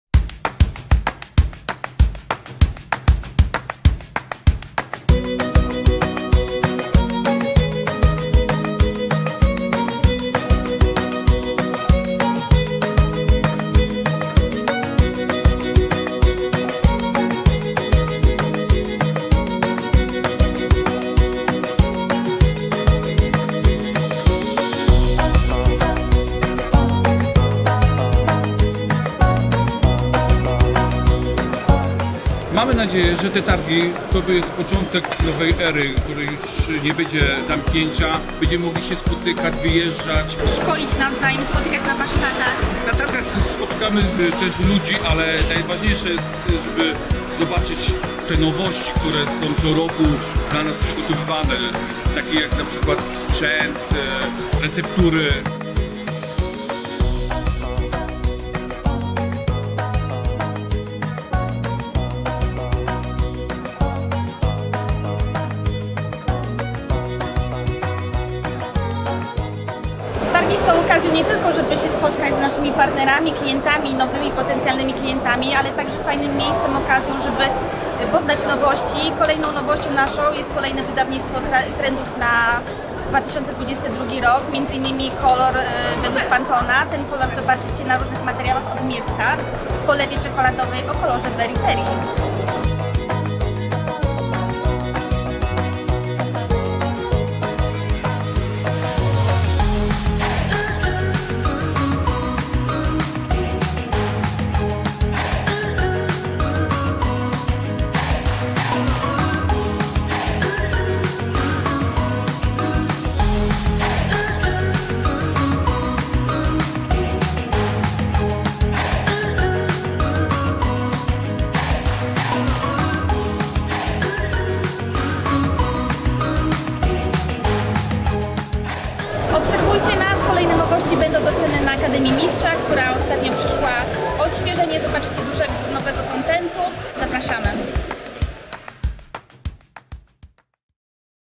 Bunge Polska (dawn. Kruszwica) na Expo Sweet 2022 - relacja z targów
Za nami jedno z najważniejszych wydarzeń w polskim kalendarzu cukierniczym. Po rocznej przerwie znów mogliśmy spotkać się na Targach Expo Sweet!
Zachęcamy do obejrzenia najciekawszych migawek z tego inspirującego wydarzenia.